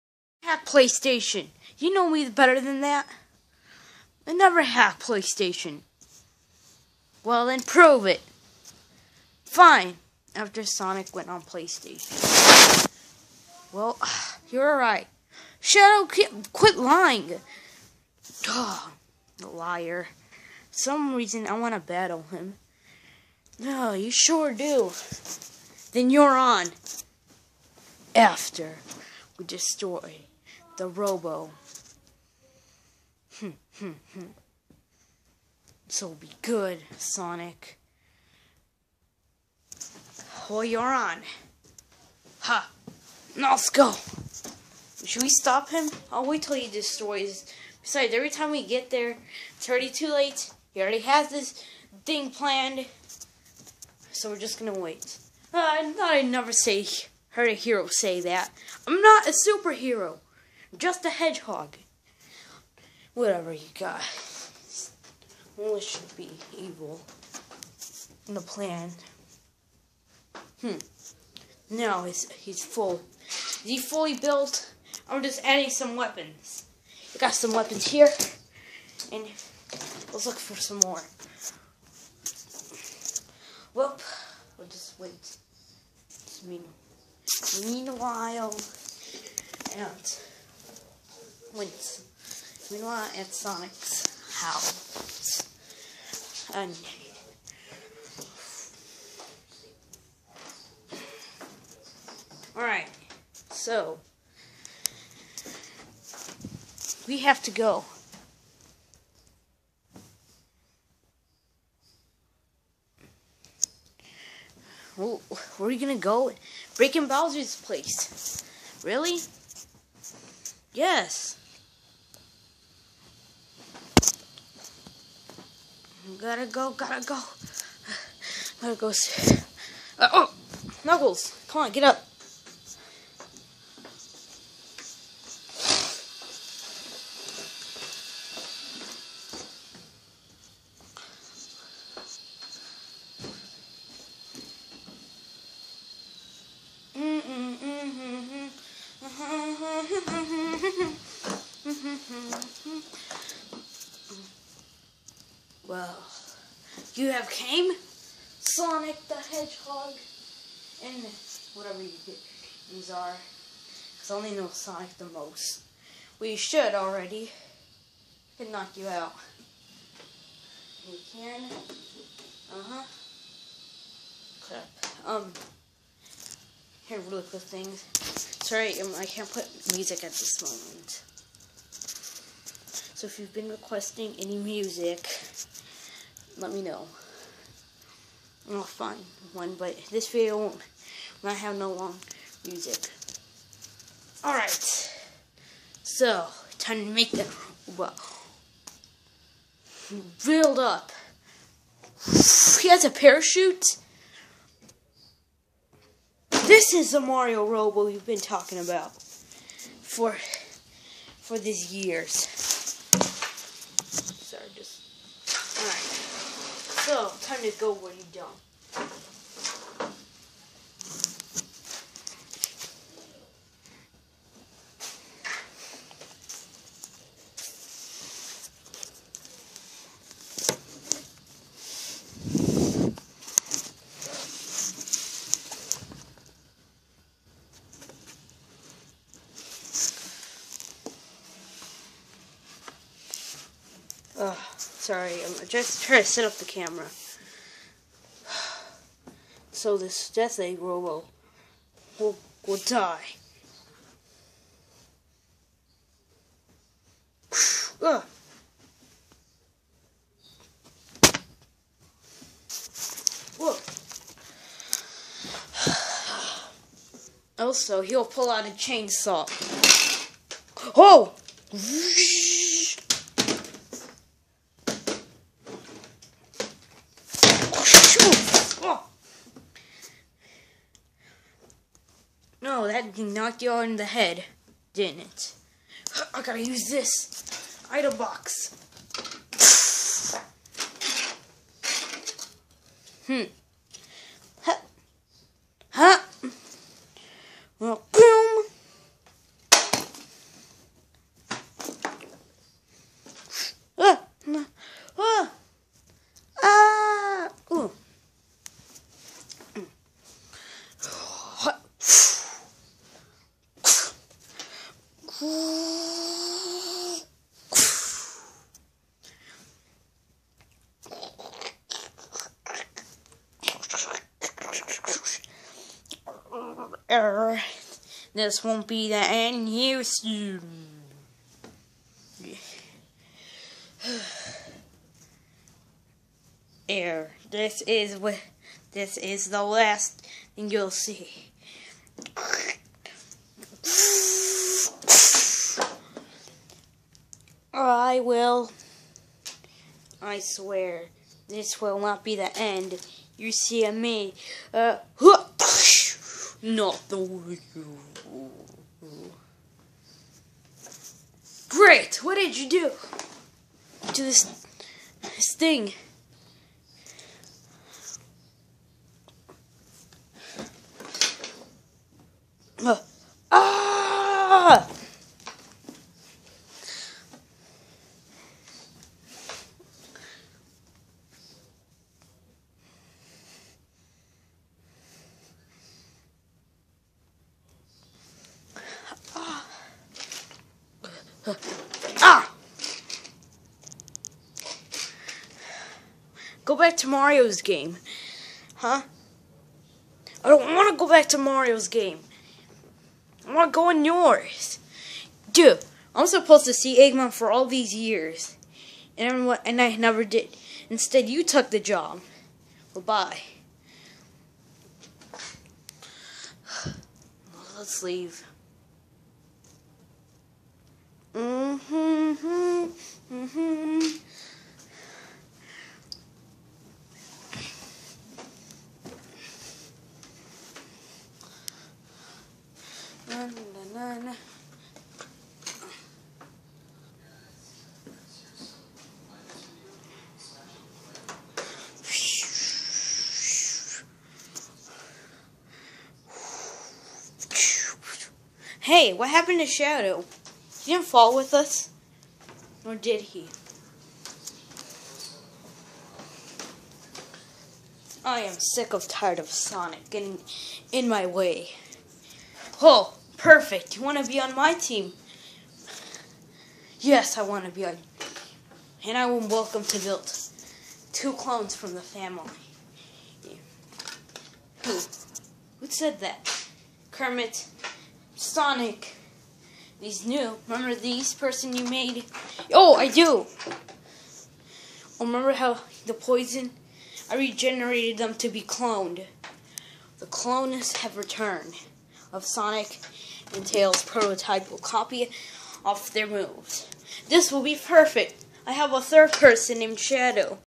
Sonic heroes epic adventures episode 7 pt.2
The heroes fight the
Mario robo